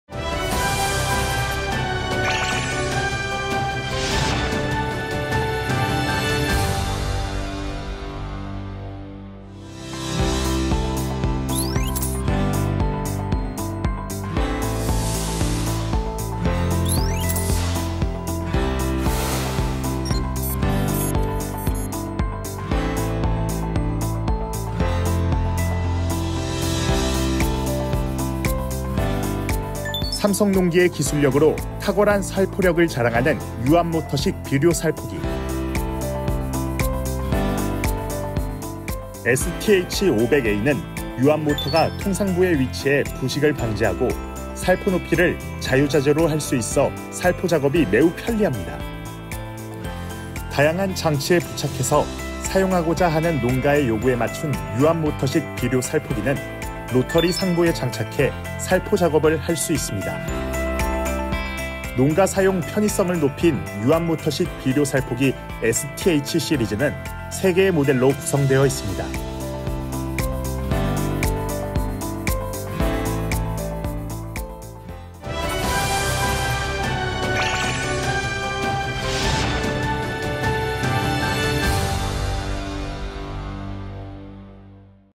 삼성농기_유압식비료살포기
삼성농기의 기술력으로 탁월한 살포력을 자랑하는 유압모터식 비료살포기.

STH 500A는 유압 모터가 통상부에 위치해 부식을 방지하고
살포높이를 자유자재로 할 수 있어 살포작업이 매우 편리합니다.
다양한 장치에 부착해서 사용하고자 하는 농가의 요구에 맞춘 유압모터식 비료살포기는 로터리 상부에 장착해 살포 작업을 할 수 있습니다.

농가 사용 편의성을 높인 유압모터식 비료살포기 STH 시리즈는 3개의 모델로 구성되어 있습니다